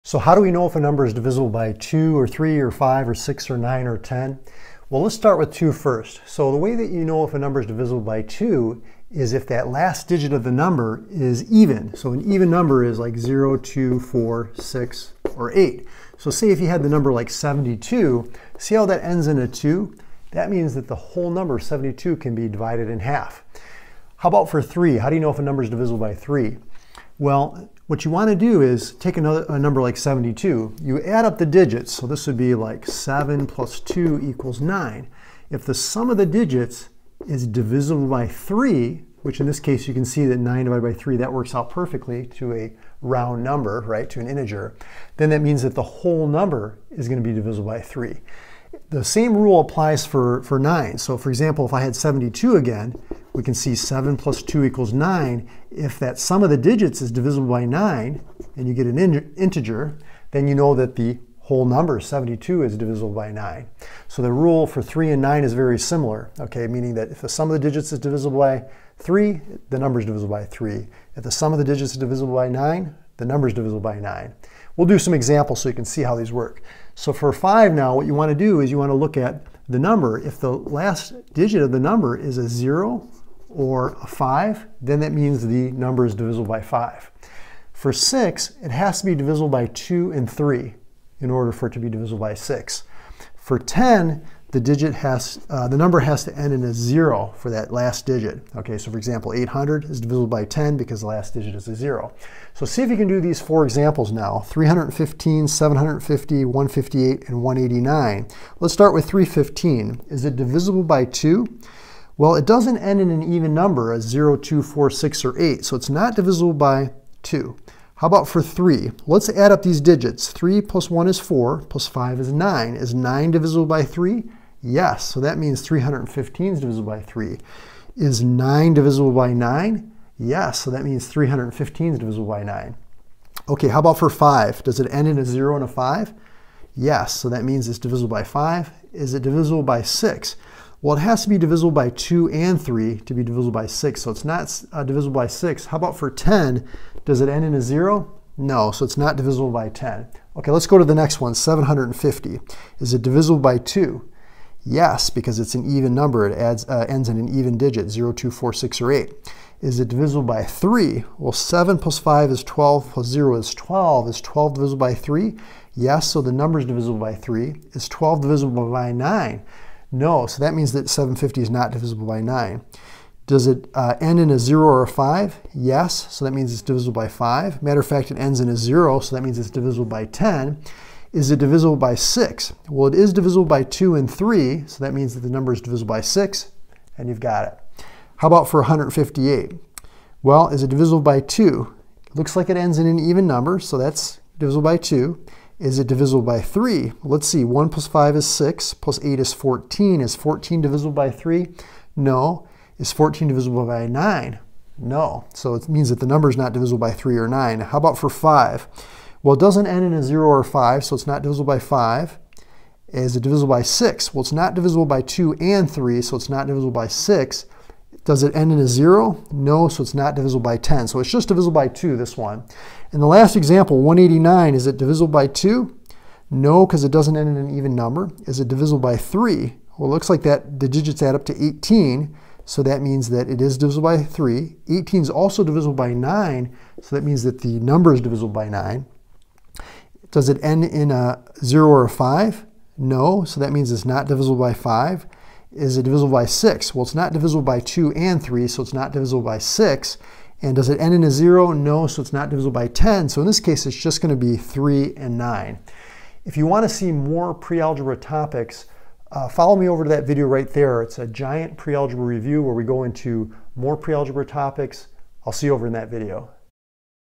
Divisibility Rules For 2,3,5,6,9,10
Learn about the Divisibility Rules For 2,3,5,6,9,10.  We discuss the rules as well as go through 4 examples to help you understand how to apply them in this video math tutorial by Mario's Math Tutoring.